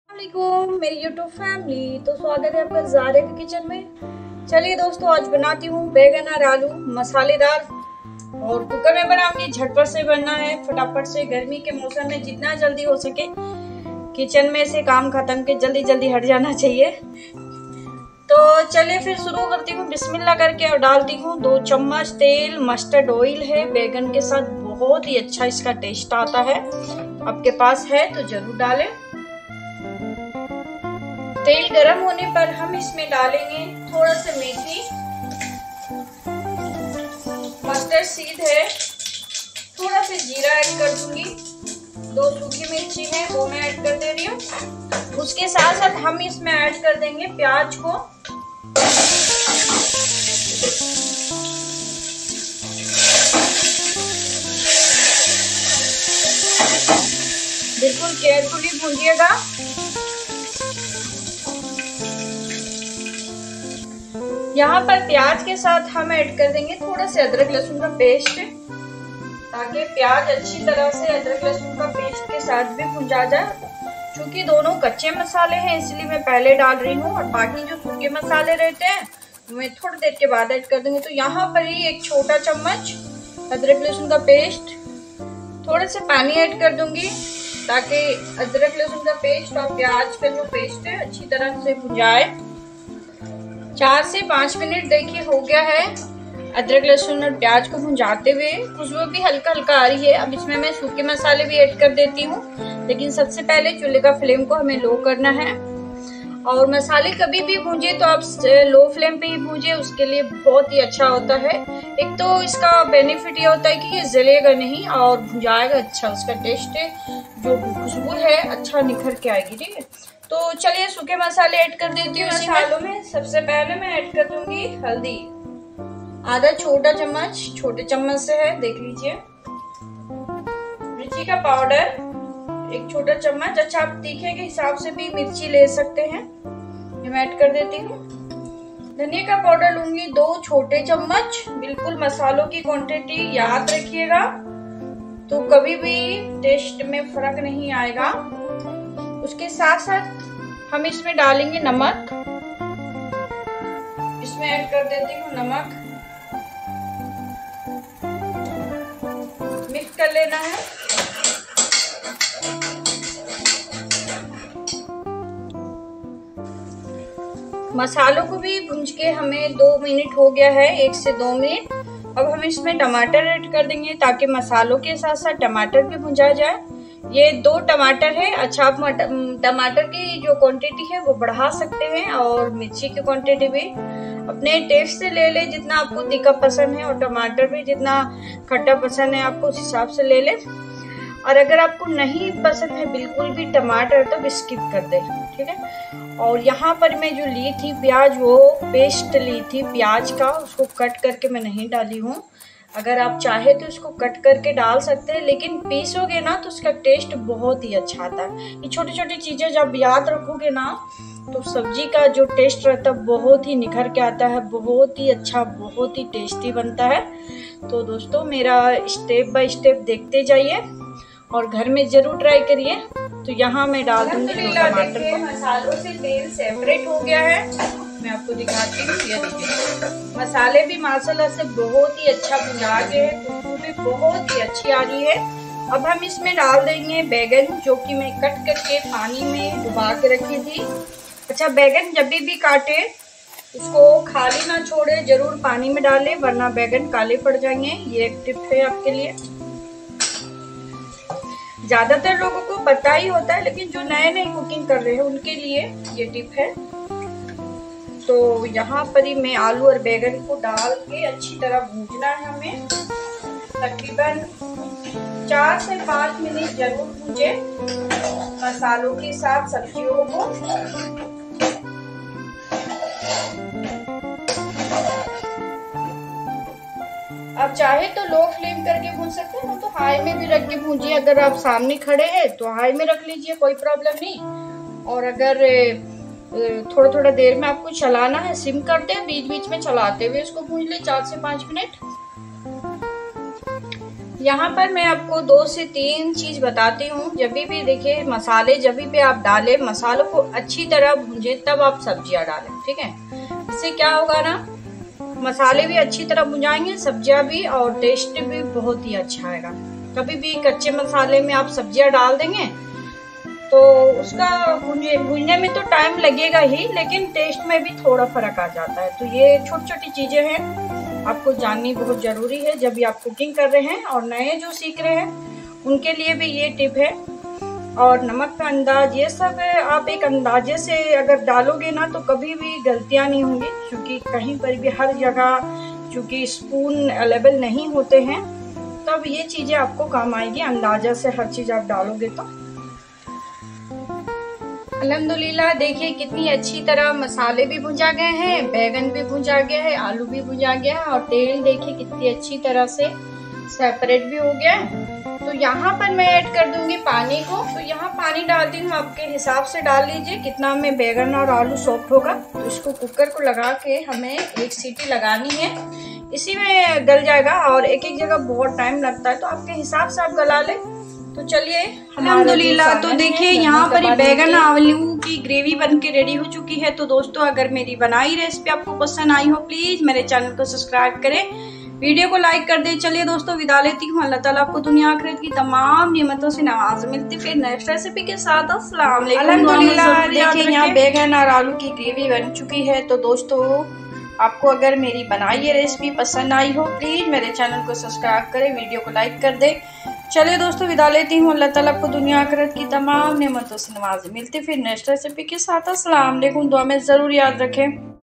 Baigan aalu ki gravy banane ka tarika
Baigan aalu ki gravy banane ka tarika # gravy

Baigan 1 pow
Aalu 1 pow
Mustard oil 2 chota chummch
Meethi
Sarso
Jeera
Pyaj I
Adrak lahsun peast I chota chummch
Haldi powder 1/2 chota chummch
Mirchi powder 1 chota chummch
Daniya powder 2 chammach in
Tamatar 2